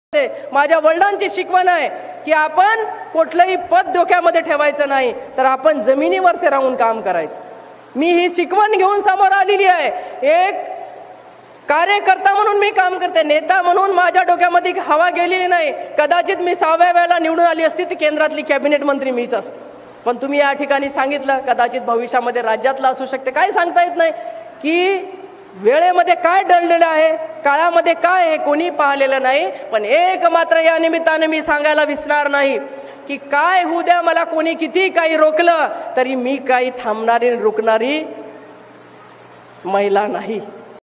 सहाव्यांदा निवडून आले असते तर कॅबिनेट मंत्री मीच असते, कदाचित राज्यात मंत्रिपद मिळणारअसेल भावना गवळी
आ.भावना गवळी यांनी नांदेडमध्ये केले वक्तव्य. Nanded Bhawna Gawli